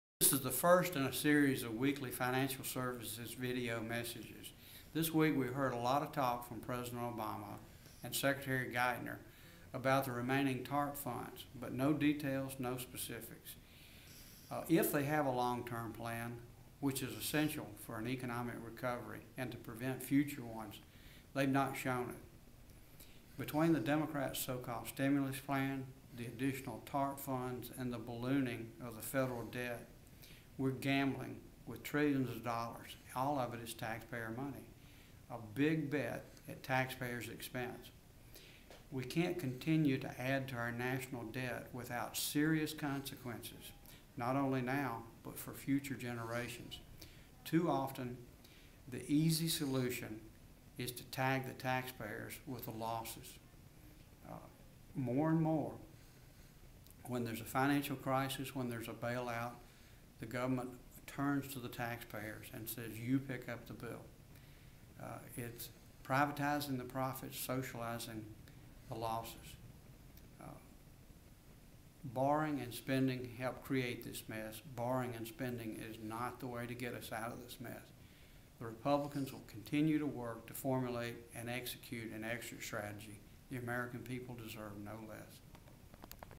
Bachus Weekly Video Message
Ranking Member Bachus delivers the Committee's Weekly Video Message. 2-13-09